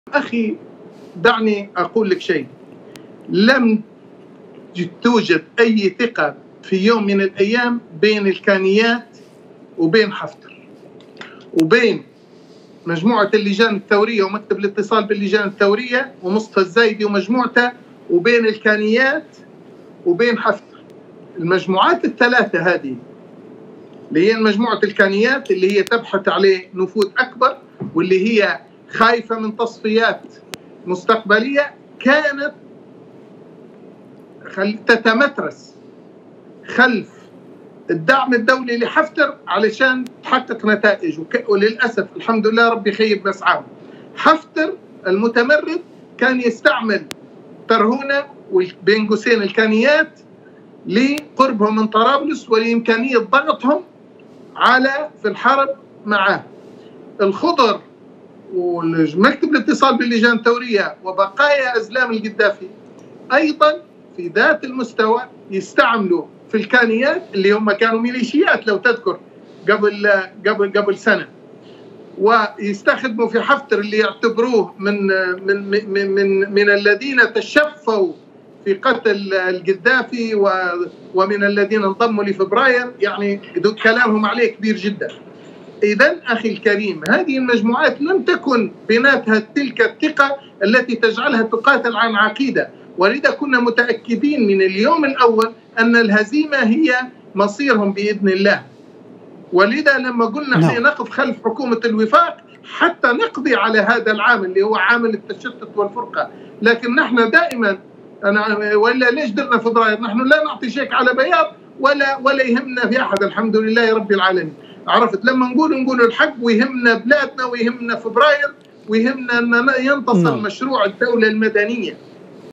محمود عبدالعزيز : لا ثقة بين الكانيات و حفتر و أتباع مكتب الاتصال باللجان الثورية
من تغطية خاصة | محمود عبدالعزيز/ محلل سياسي : لا ثقة بين الكانيات و حفتر و أتباع مكتب الاتصال باللجان الثورية | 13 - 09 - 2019

ــــــــــــــــــــــــــــــــــــــــــــــــــــــــــــــــــــــــــ
ــــــــــــــــــــــــــــــــــــــــــــــــــــــــــــــــــــــــــ
يمكنكم تحميل تطبيقنا TANASUH TV على الهواتف الذكية: